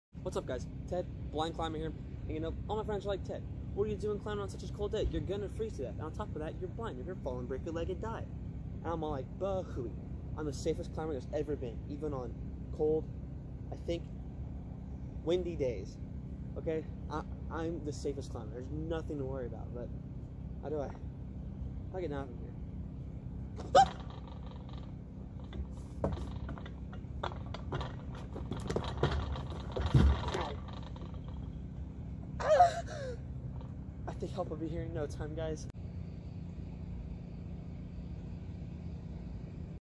Ted Blind Climber
Ted the Blind Runner goes climbing